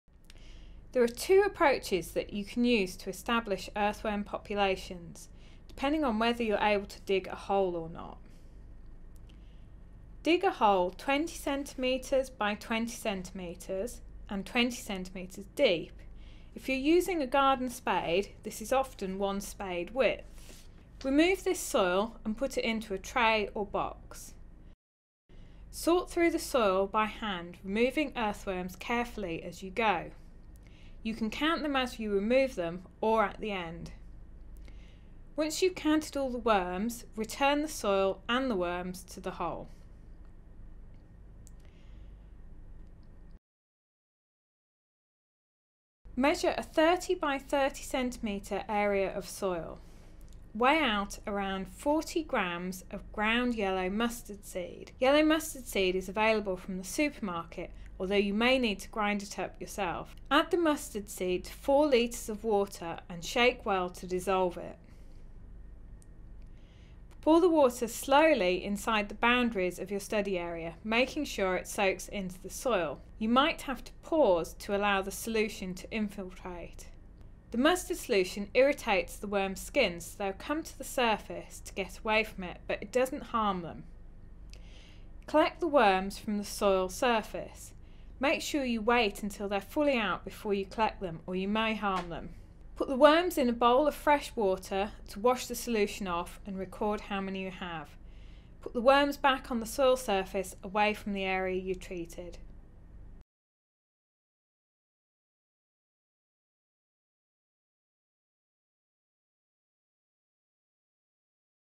How to find out how many worms there are in the soil
‘Soils: Introducing the World Beneath Our Feet’ is a free online course by the University of Lancaster available on FutureLearn.com

Earthworms are important for soil formation and health but how many worms are there in the soil? Watch this video to see two methods for estimating worm numbers.

Please take care whilst digging and handling soil:

- You will be handling soil. Be wary of sharp objects that may be buried and not immediately visible
- If you are using a shovel, make sure you are wearing shoes (not barefoot or sandals)
- Be aware of any biting insects (or other creatures) that may be living in the soil or undergrowth
- Wash your hands when you have finished